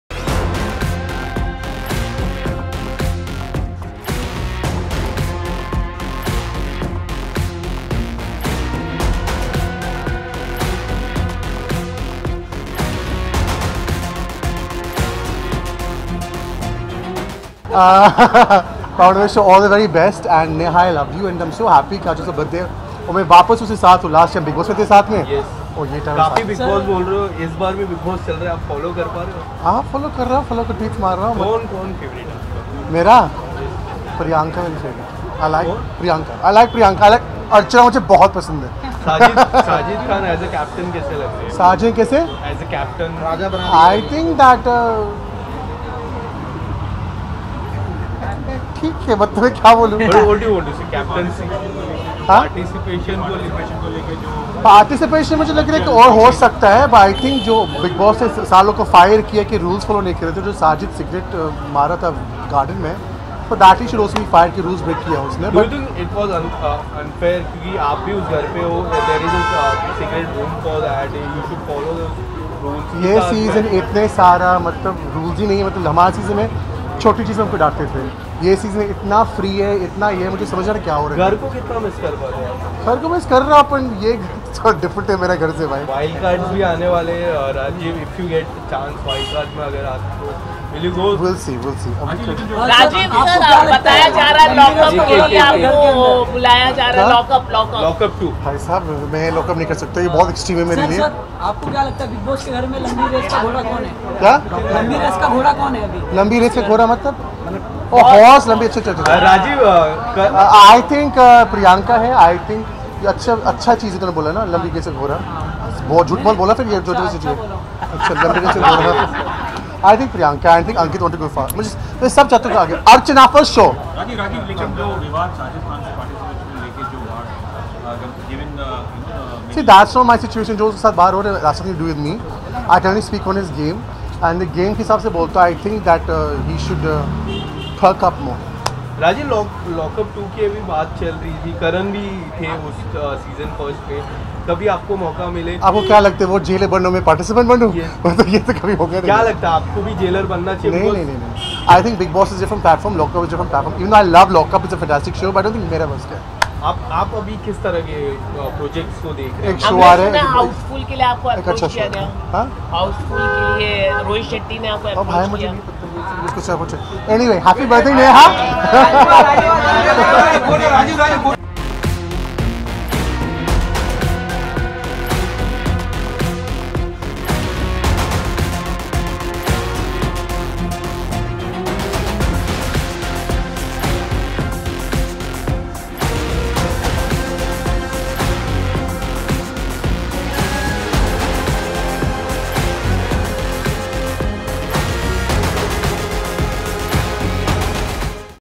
Rajiv Adatia shows STRONG support to Archana Gautam and Sajid Khan's captaincy in Bigg Boss 16
Rajiv Adatia attended Neha Bhasin's birthday where he revealed who is his favorite Bigg Boss contestant and what he thinks of Sajid Khan as a captain. He also said that this season is quite different from others. Watch the video to know more!